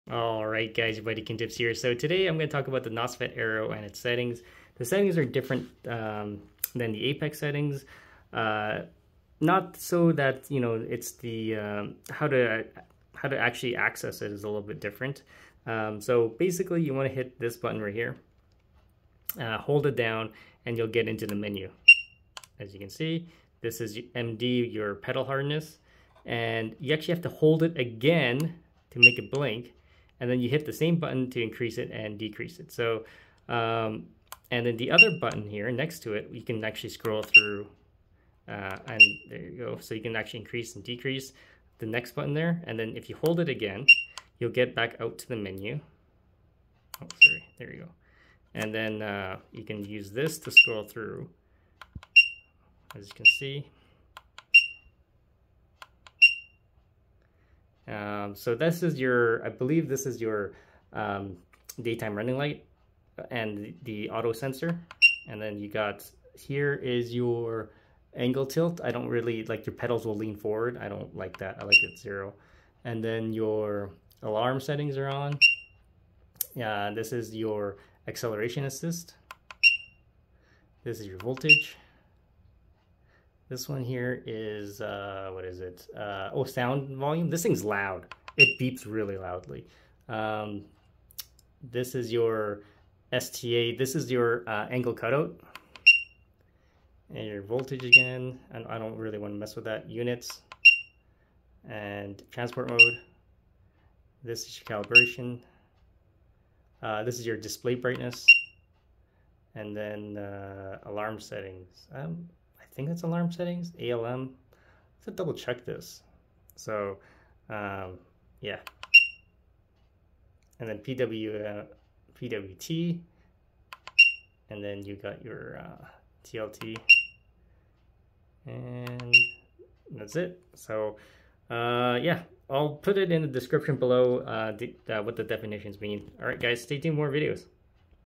How to adjust your Nosfet AERO settings and what they mostly mean EUC Electric Unicycle
-there's 3 new settings not found on the APEX, that are on this wheel.

Settings:
MD-Ride mode-pedal hardness
ANG-Vertical Angle adjustment
PWT-PWM value
ALM-Alarm speed
ALM-torque alarm
BRT-brightness screen
BRT-Auto light
CAL-Calibration
TRM-Transport mode
UNT-Units KM or MPH
VtA-Voltage adjustment
StA-Slide angle adjustment also know as "angle cut off"
MxV-Max voltage
ANG%-acceleration assist
ALM-alarm when max torque achieved
ANG TLT-angle tilt recentre